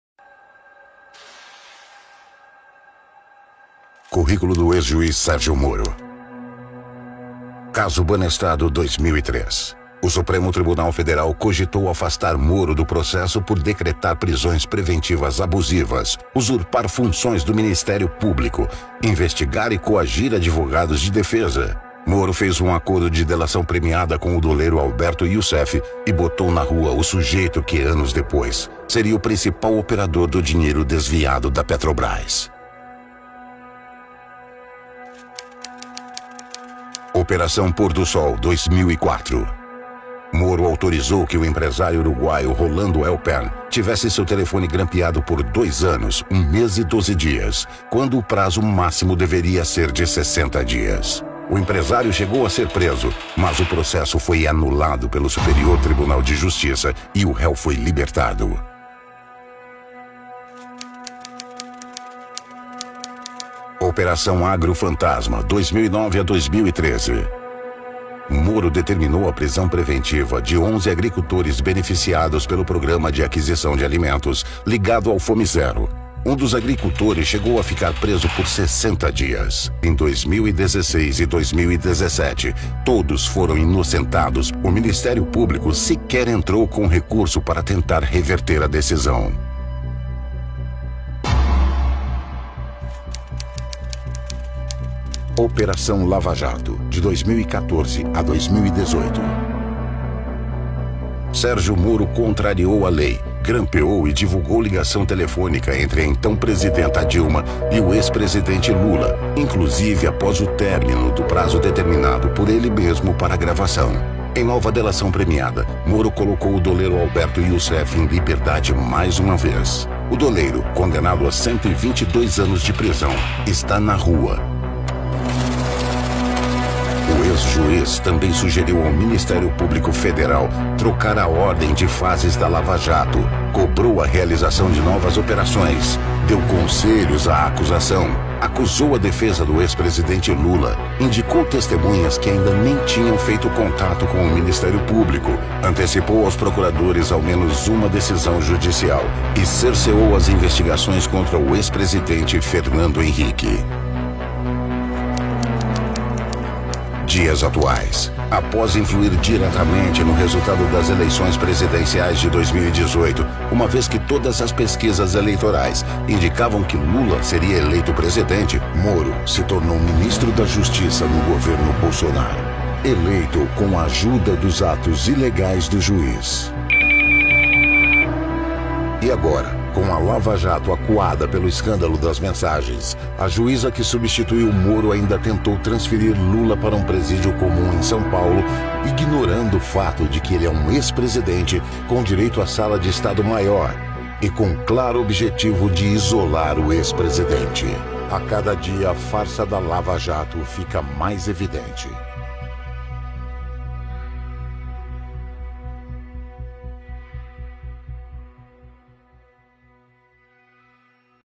Eis o currículo de Sergio Moro: um juiz parcial
Eis o currículo de Sergio Moro: um juiz parcial, com decisões questionáveis em várias oportunidades, um ministro incapaz, que defende que policiais tenham o direito de matar, e que agora terá R$ 10 milhões para fazer propaganda de um projeto ruim.